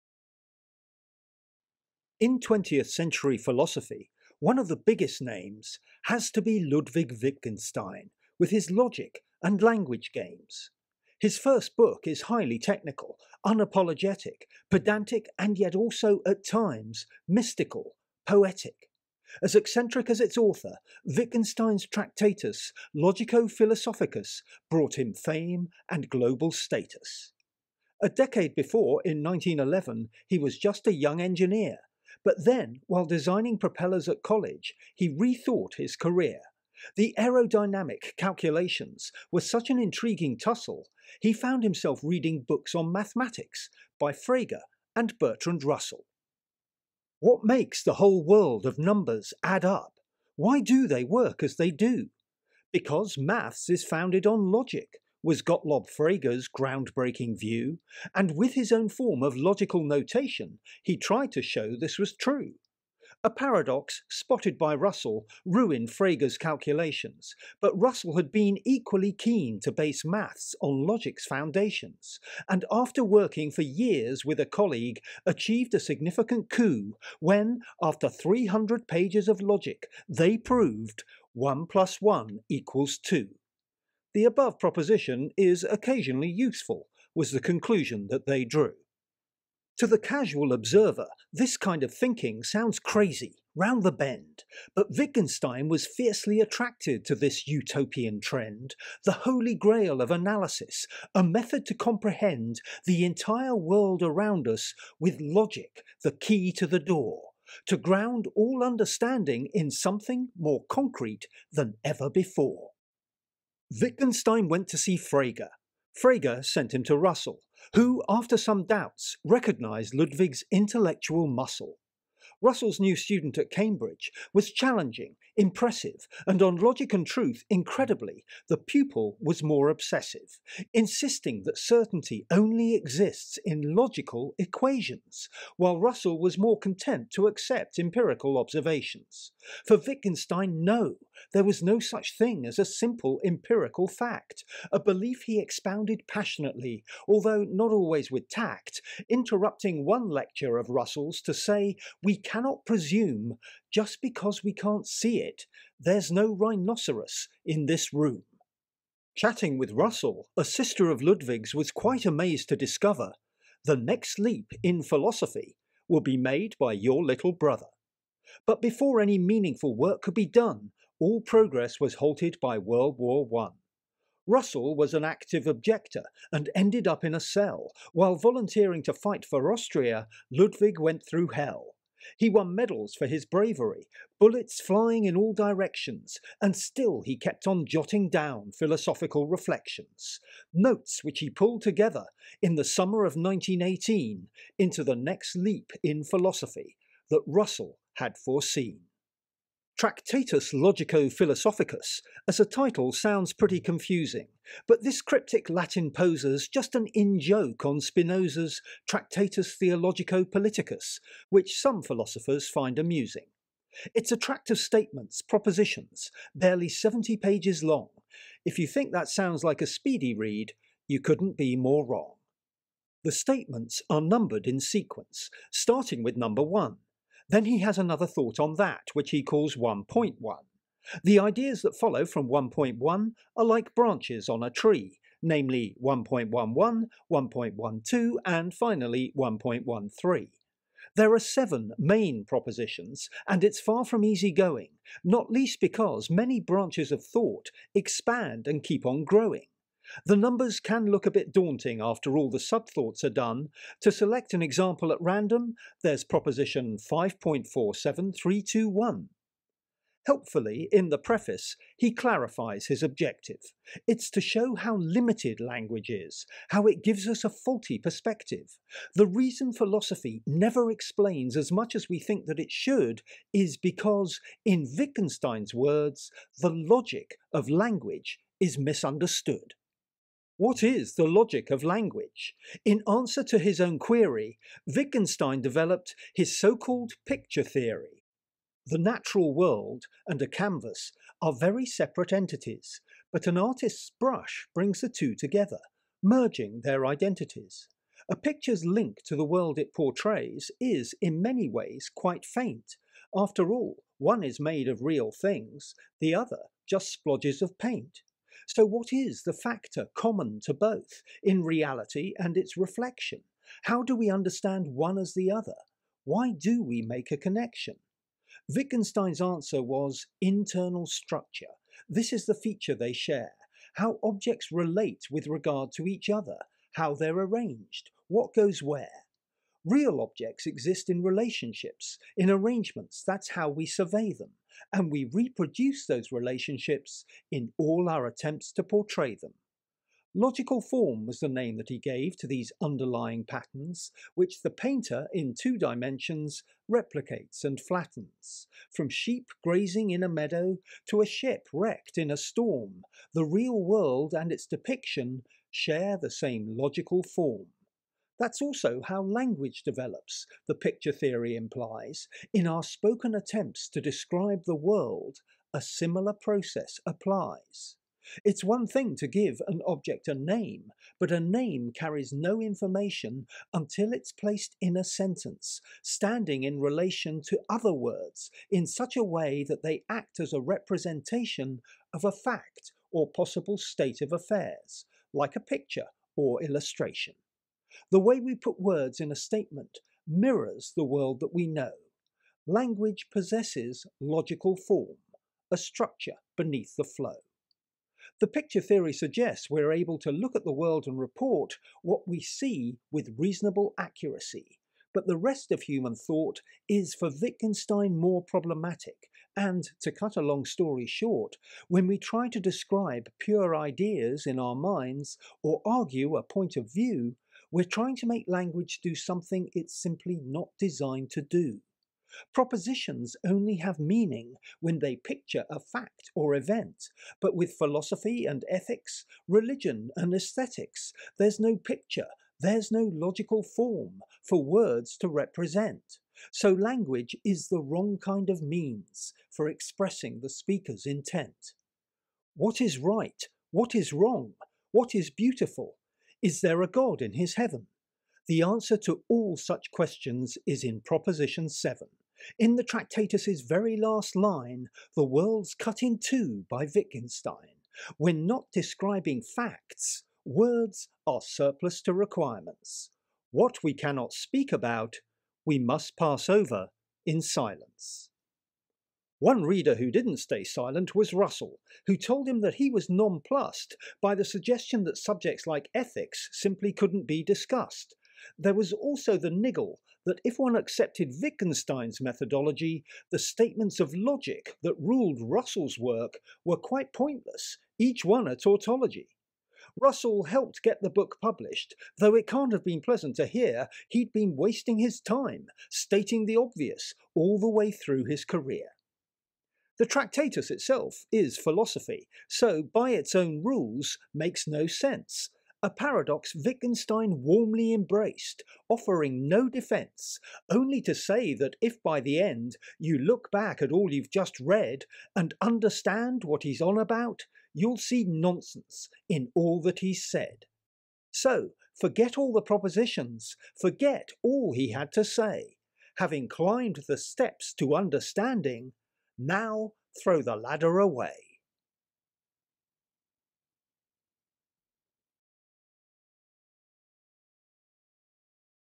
Wittgenstein v Russell: Tractatus Logico-Philosophicus in verse! The Picture Theory and more!
A brief overview of Ludwig Wittgenstein's Picture Theory of language, as shown in the Tractatus, his famously cryptic book of logical propositions.  Includes the inspiration of Gottlob Frege and Bertrand Russell, the concept of logical form - and a rhinoceros!